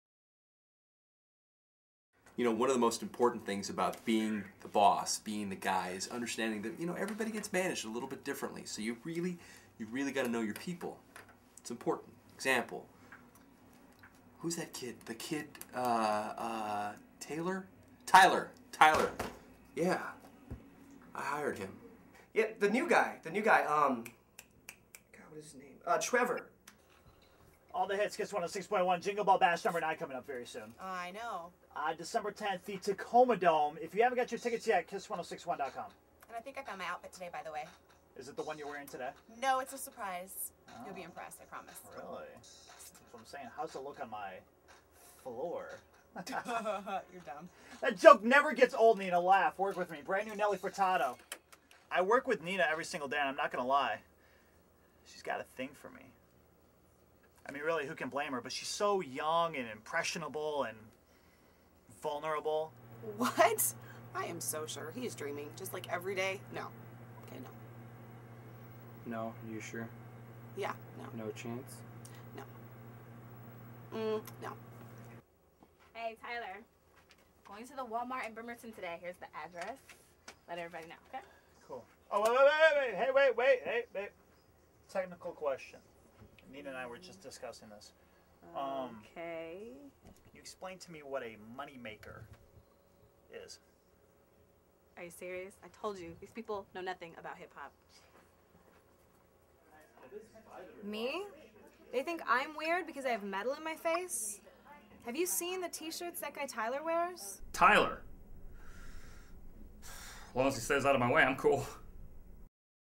KISS 106.1- The Studio Part 3
- KBKS KISS 106.1 in Seattle did a spoof on "The Office" called The Studio...check it out! Video created by Riley and Nick!